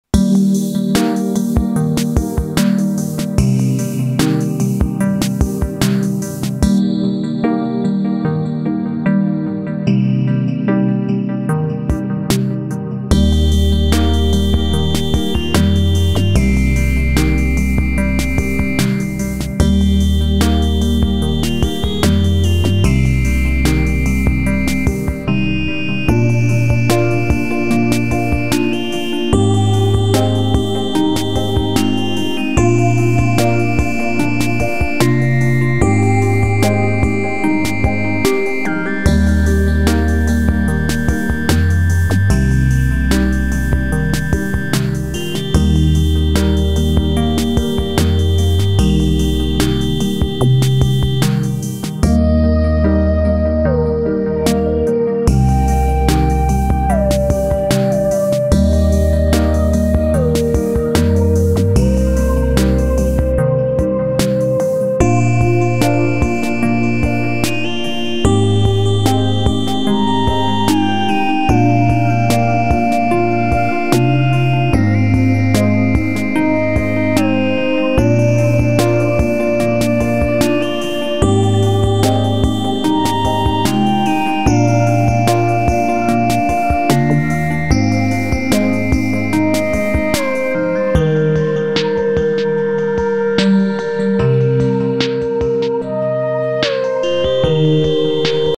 Playinginthesnow
My kids here are having a good time playing with the snow.